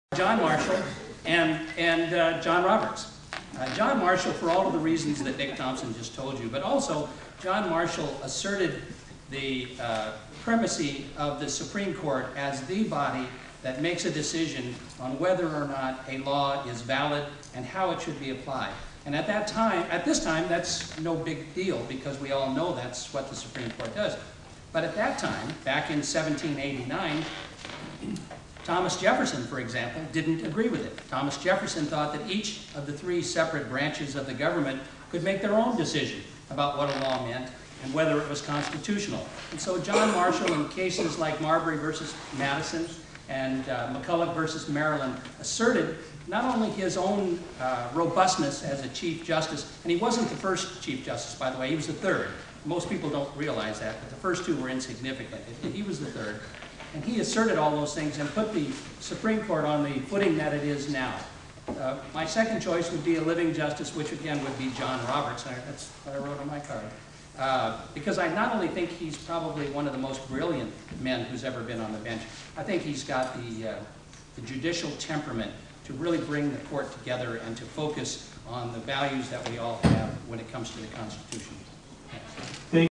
Judges forum: Mike Bartlett part 2 - 2008-04-05
Mike Bartlett discusses Supreme Court justices and local judges whom he admires at the judges forum. Published on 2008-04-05